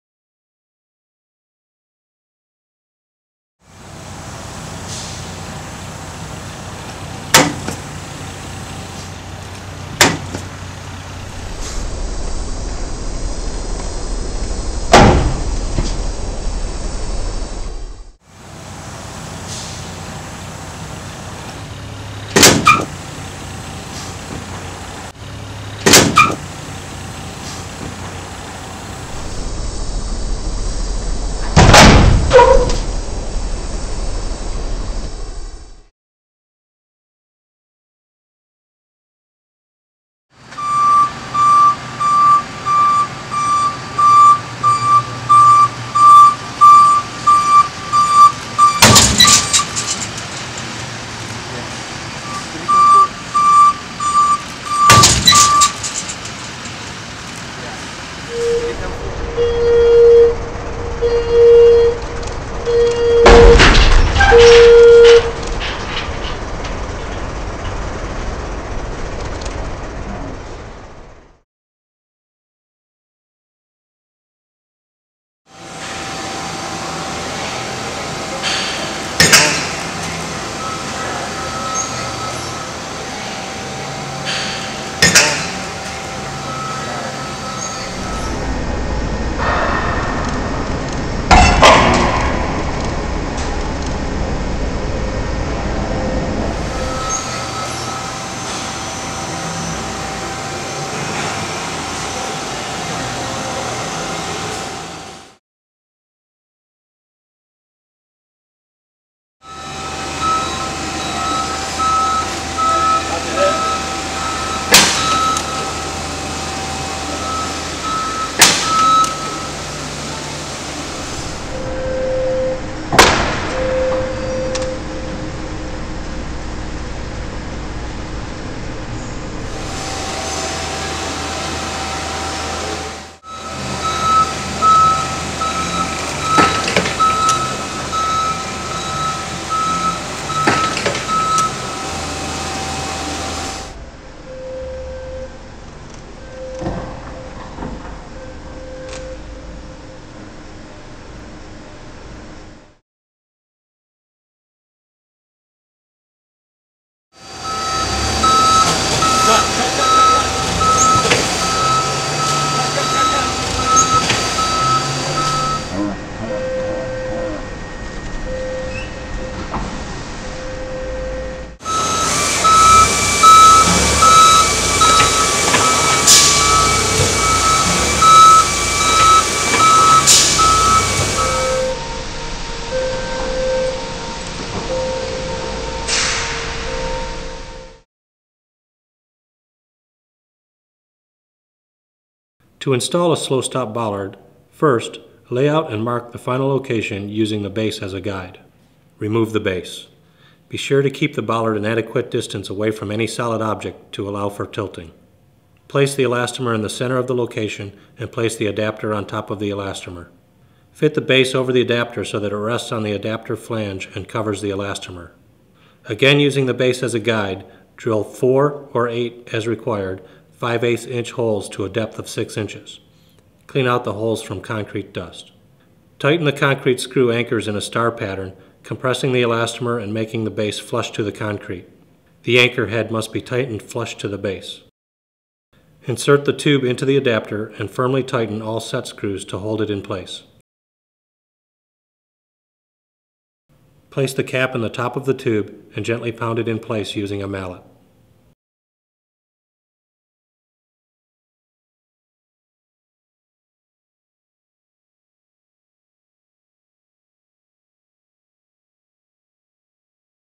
SlowStop Bollard Demonstration and Installation Video
Demonstration of use of the revolutionary new SlowStop Bollard system.  Prevents building and asset damage while protecting pedestrians, vehicles, and loads.